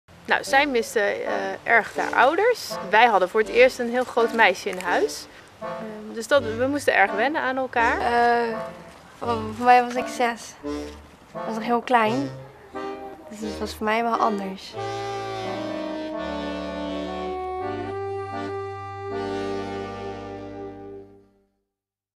Caitlin
Soms zijn ouders niet in staat om voor hun kinderen te zorgen, vanwege moeilijk gedrag van een kind of andere problemen thuis. Een tijdelijke woonplek in een ander gezin kan dan uitkomst bieden.